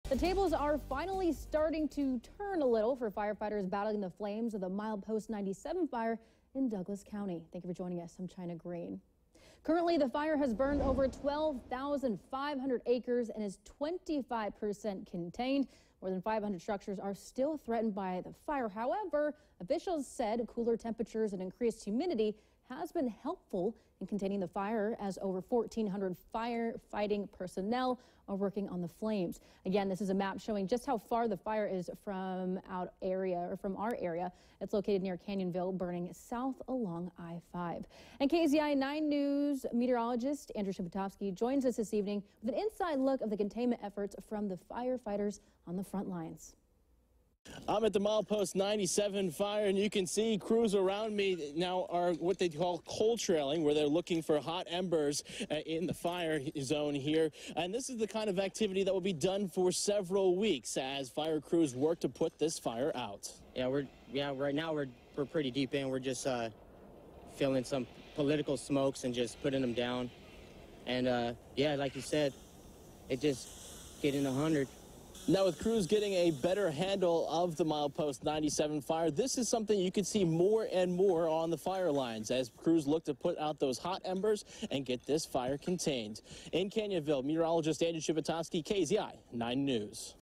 Crews starting to mop up Milepost 97 Fire
Firefighters are looking to extinguish embers in the fire zone.